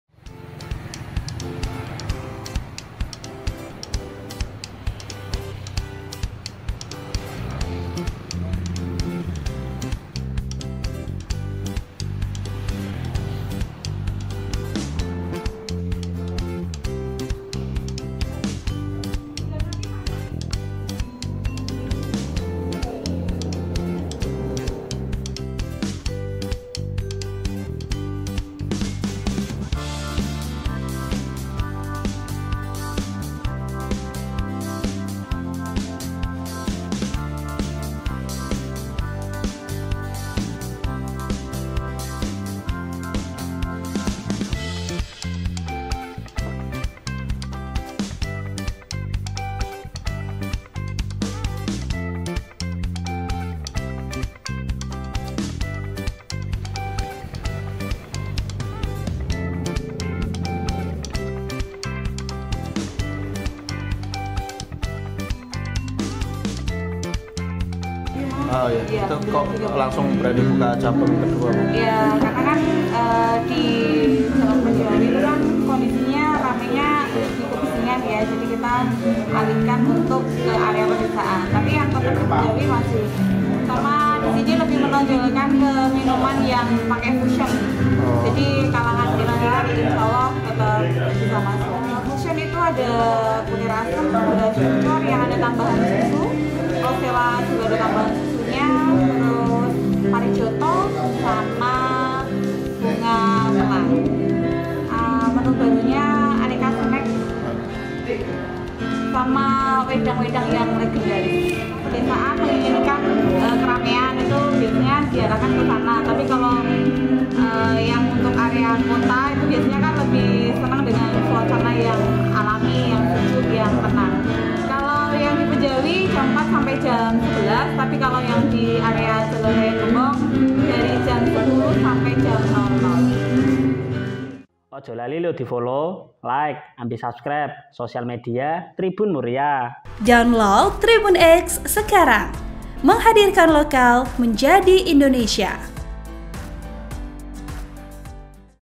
Sensasi Baru Minuman Fusion Rempah di Teras'e Jeng Minul Margorejo Pati
Tren minuman rempah di Kab Pati tengah melejit.

Satu di antaranya ialah Teras'e Jeng Minul milik Indah Susanti.

Teras'e Jeng Minul di Jalan Raya Pati-Gembong, menawarkan suasana lebih tenang, sejuk, dan alami.

Beberapa menu baru andalan di antaranya ialah gula asem sparkling.

Menu ini merupakan perpaduan minuman gula asem dengan air lemon serta topping karamel.

Cafe Rempah Teras'e Jeng Minul di Jalan Raya Pati-Gembong buka setiap hari mulai pukul 10.00 sampai 00.00 WIB.

Editor: edr